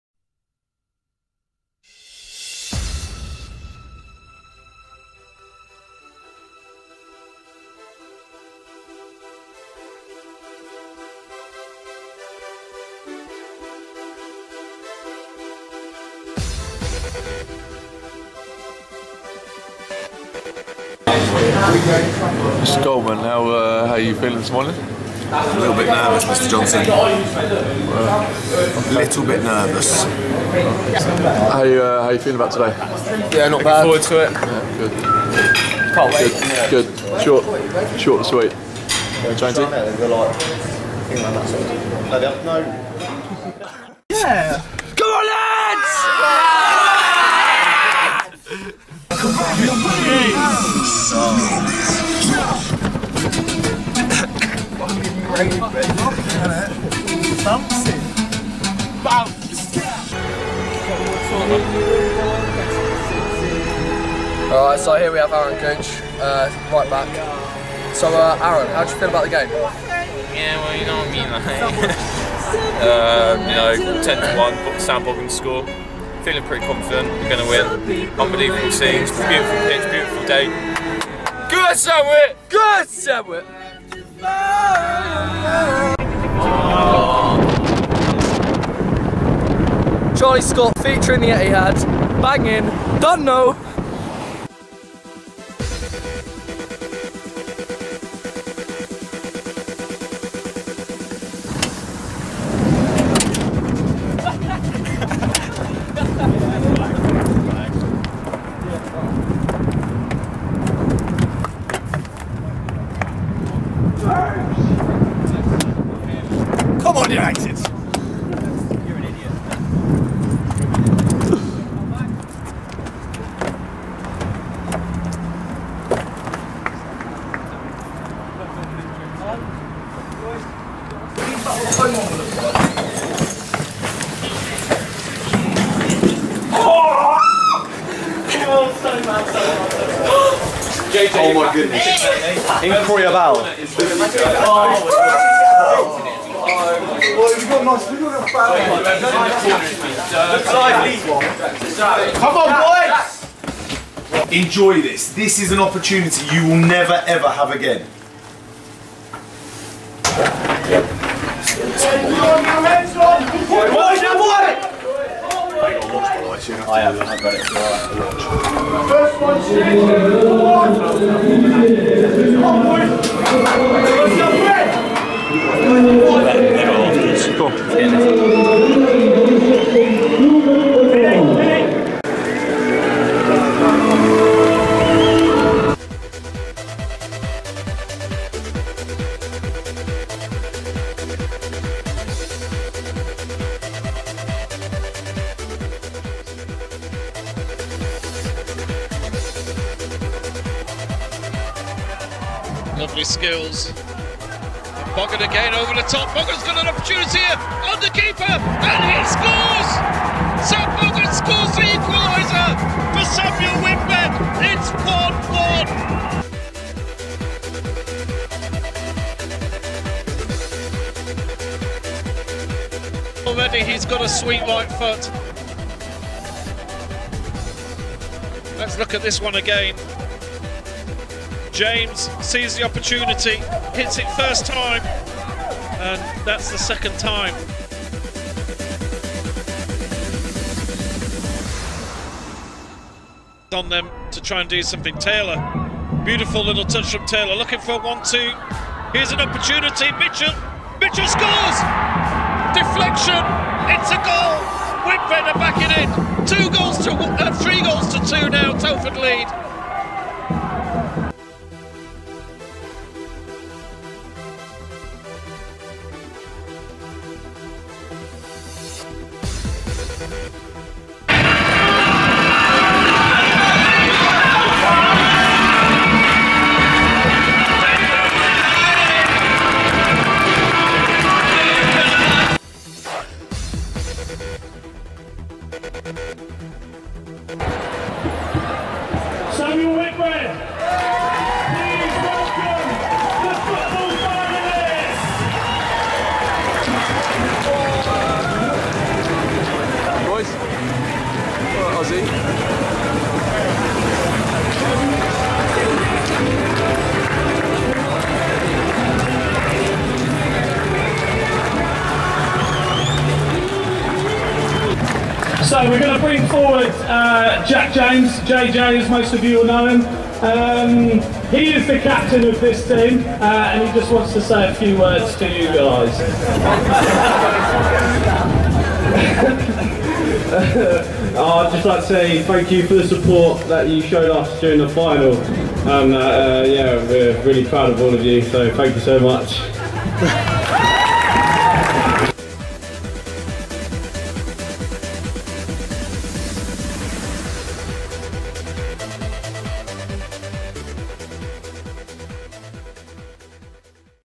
SWA U16 Football Boys National Final Video Diary 2016
SWA U16 boys footballers become the first Whitbread side in history to reach a national football final.